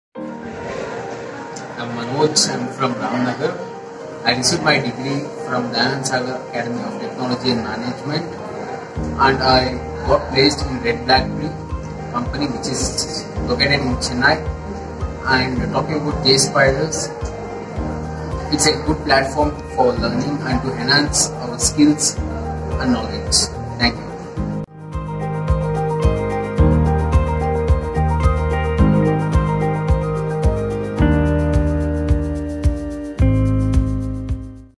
ASSOCIATE SOFTWARE ENGINEER | Student's Experience and Feedback | Jspiders Basavanagudi
Congratulations MANOJ.S for getting placed as ASSOCIATE SOFTWARE ENGINEER

Do use this opportunity for your wonderful career start..!!
Attend Free Demo classes..

CONTACT SOON !!